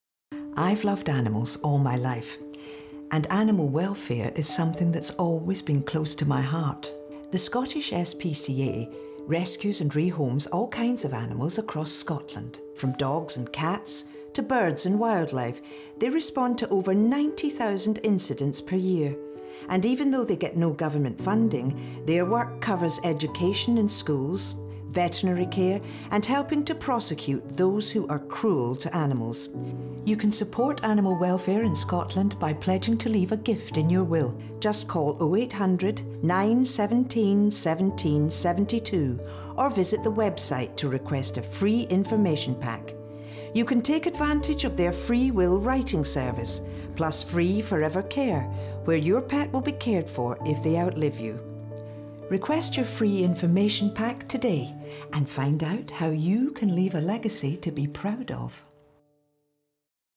Leave a legacy with the Scottish SPCA
Phyllis Logan, star of Downton Abbey, explains how you can let your love of animals live on and on by leaving a gift in your Will to the Scottish SPCA.